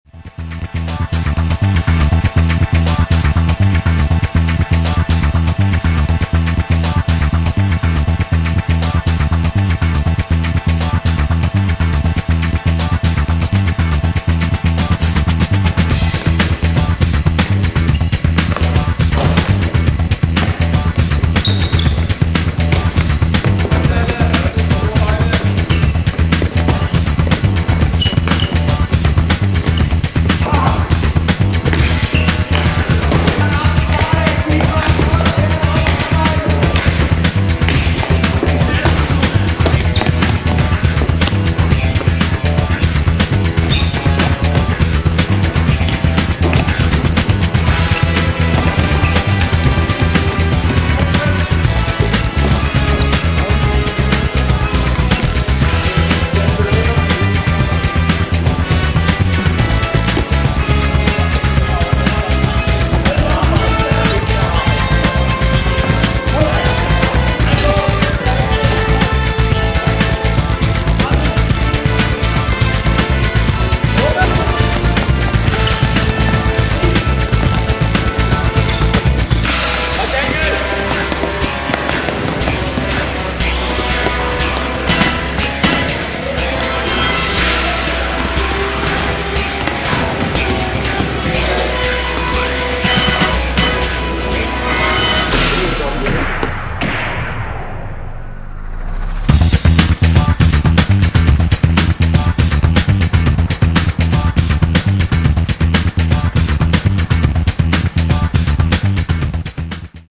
Rungsted HK trailer
Kom en tur med i Rungsted-Hallen og se de lokale floorball-drenge træne. Klik ind på vores hjemmeside og læs om vores træningstider.